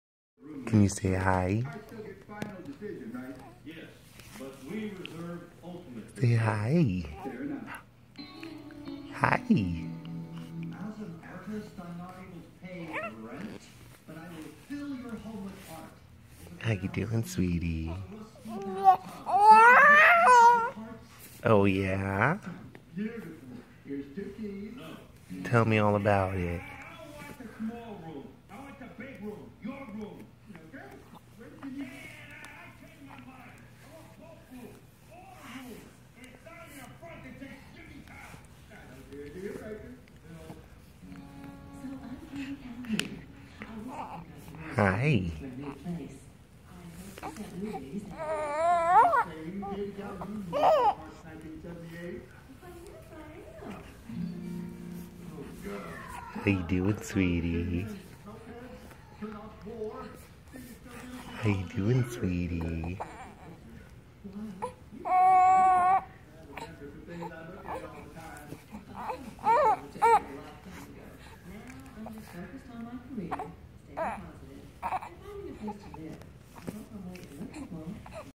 Our baby girl, pouncing herself in the face!
A cute two month old hanging out with her daddy